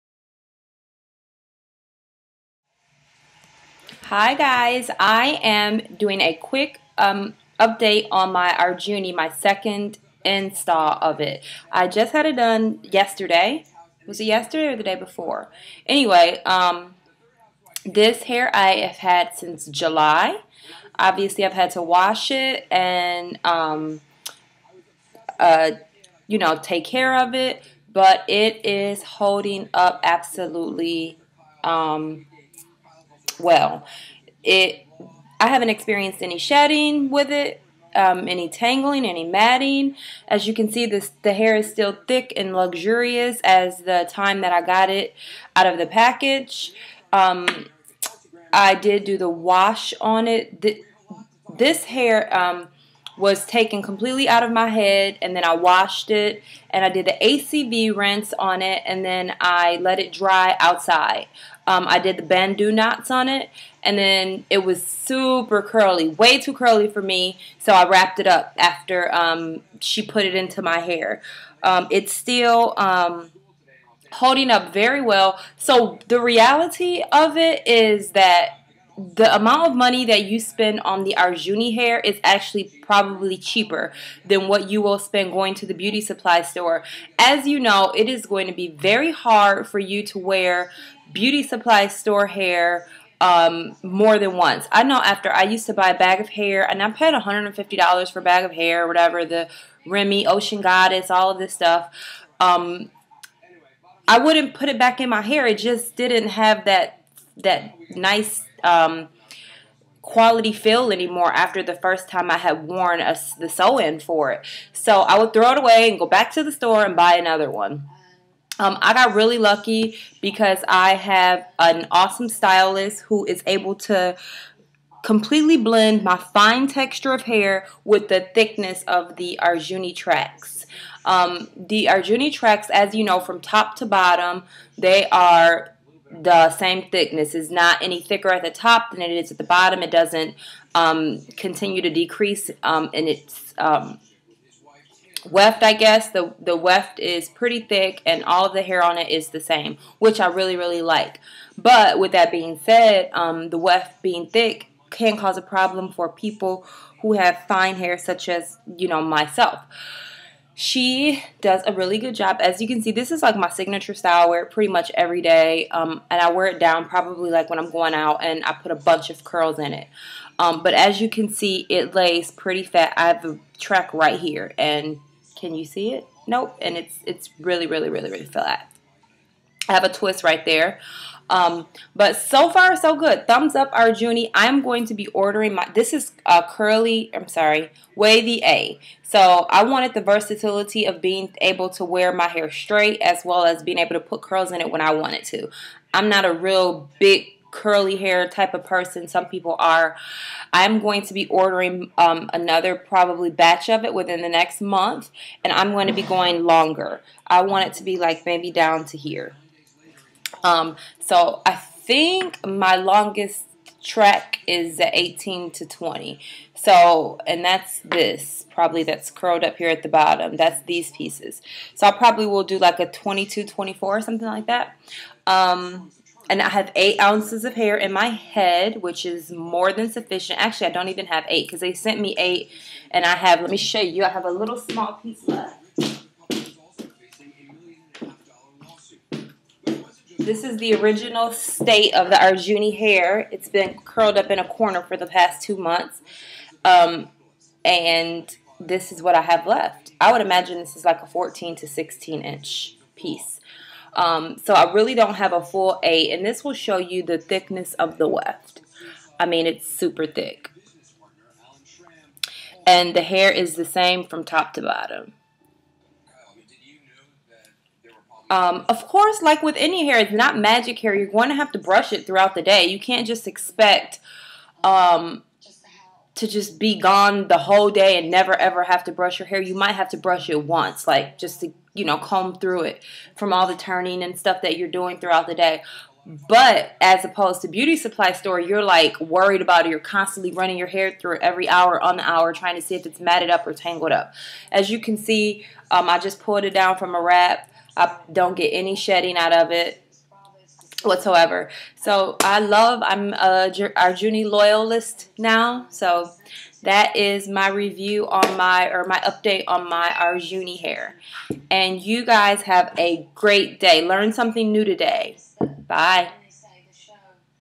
Ajuni 2 Month Update Re-Install
My Arjuni 2 month Update. My hair was installed for 2 months! I recently removed and reinstalled the hair.  The hair is holding up GREAT!!! No shedding, tangling, breakage! This hair is well worth every penny I paid for it!

Specs
Weft 1a: 12-14, Medium Luster, Brown, 2.4 ounces
Weft 2a: 14-16, Medium Luster, Brown, 2.2 ounces
Weft 3a: 16-18, Medium Luster, Brown, 1.5 ounces
Weft 3b: 16-18, Medium Luster, Brown, 2.2 ounces

Ordered on June 27, 2011
Received on July 13, 2011

I live in the Tampa Bay, FL area, please contact Saraya for all of your styling needs, SHE'S AWESOME!!